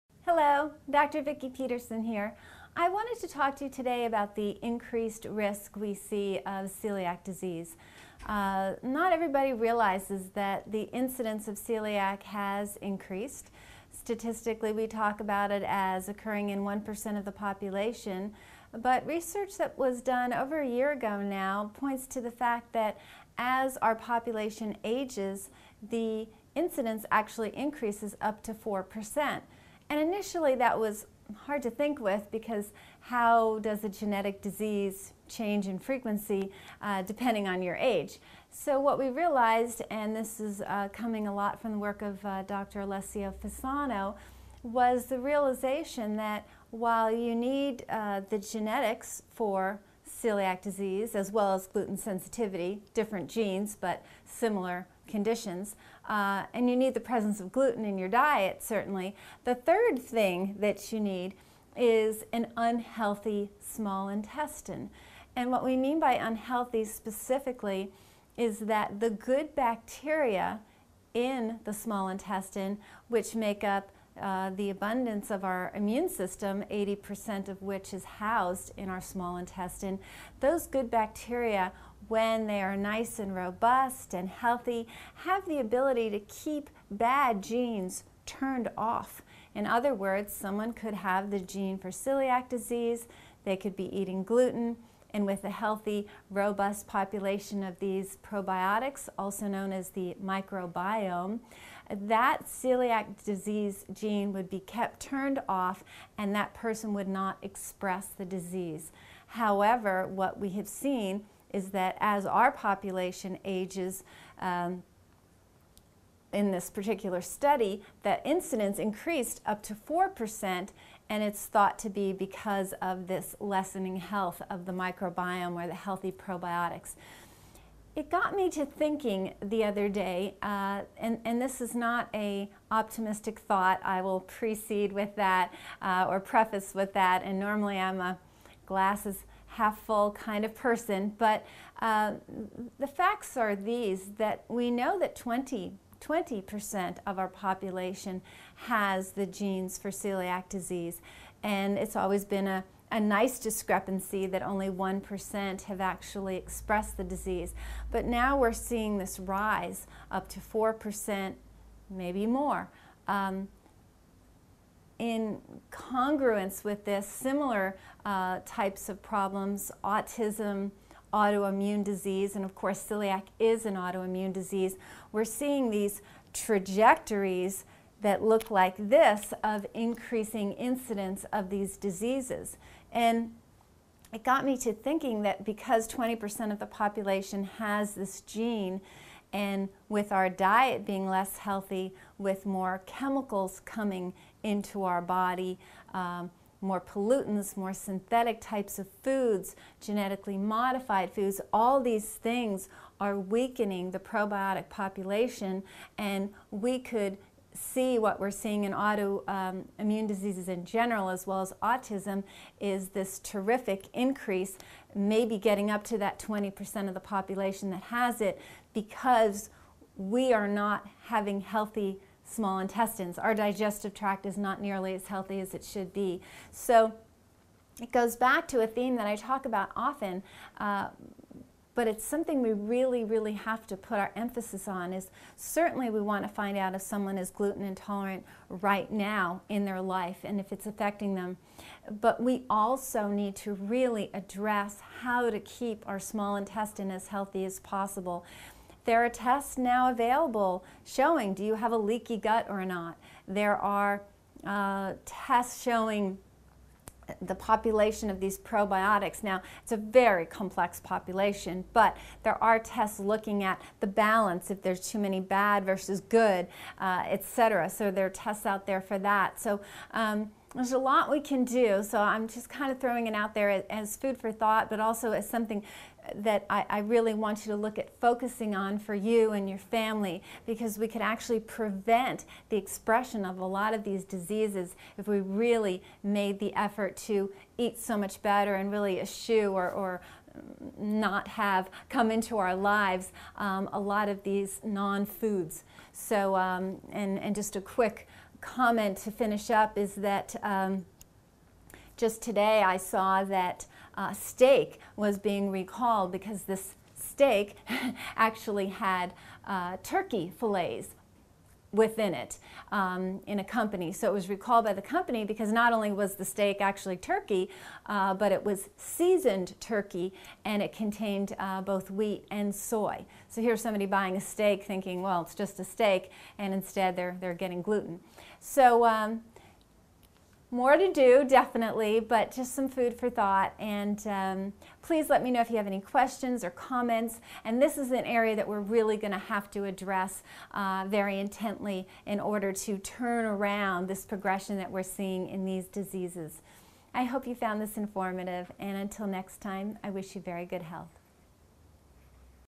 Celiac Disease: An Epidemic on the Rise
20% of the population has genes for Celiac Disease. What used to be a 'rare' disease is common at 1% and extremely common at 4% - the incidence that recent research shows. Learn more from a doctor & author.